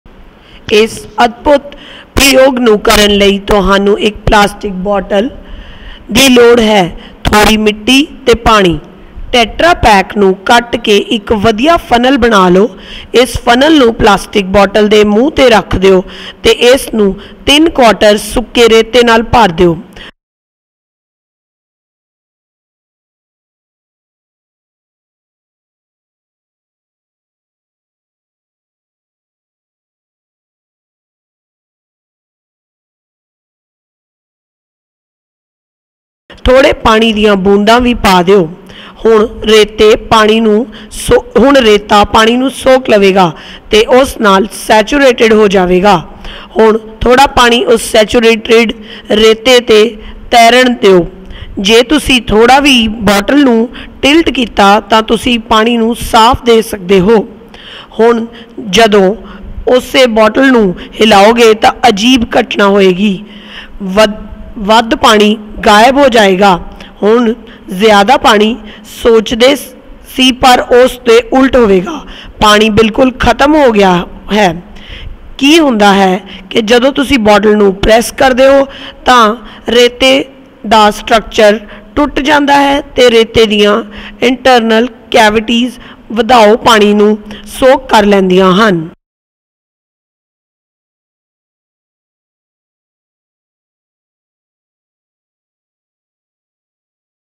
SAND SUCKS | Punjabi
For performing this very unique experiment you will just need a plastic bottle, some sand and water. Cut a Tetra Pak to make an improvised funnel. Place this funnel on the mouth of a plastic bottle and fill it three-quarters with dry and coarse sand. Keep adding little bits of water to the bottle. The sand will absorb water and soon it will be saturated with it. Let a little bit of extra water float on top of the saturated sand. If you tilt the bottle you will see the extra water clearly.
Now when you press the bottle something amazing happens. The extra water disappears!! You would expect more water to come out but the opposite happens. The water disappears. What happens is that on pressing the bottle the structure of the sand breaks and this creates internal cavities which suck the extra water!